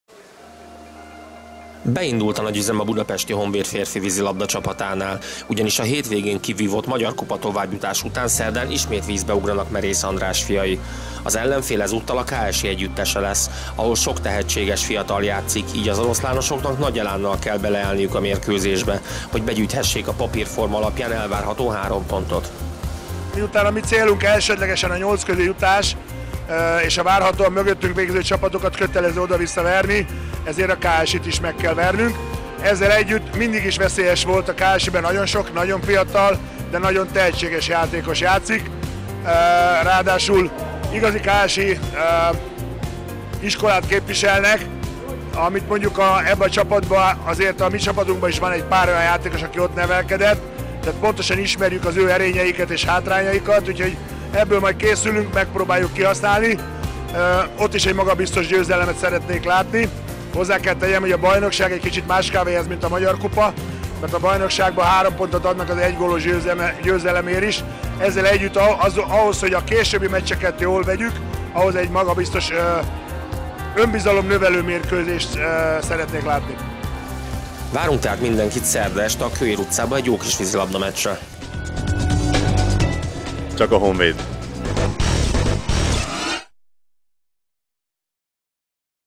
🎬Vízilabda Beharangozó – BHSE-KSI
A harmadik fordulójához közeledik a Férfi OBI Osztályú Országos Bajnokság sorozata, ahol a KSI csapatát fogadjuk hazai pályán. Mostani ellenfelünk a tavalyi OB I/B ligából az első helyről érkezett az ország 16 legjobb csapata közé és az idei szezonban egy vereséget szerzett (12-4 az Eger ellen) idáig.
Nézzétek meg ízelítőnket, hogy mi várható ezen a mérkőzésen!

Hajrá Honvéd! 💪 Hajrá Fiúk! 👏

Időpont 📅: 2018. október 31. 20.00
Helyszín🏠: Kőér utcai uszoda: 1103 Budapest, Kőér utca 2/b.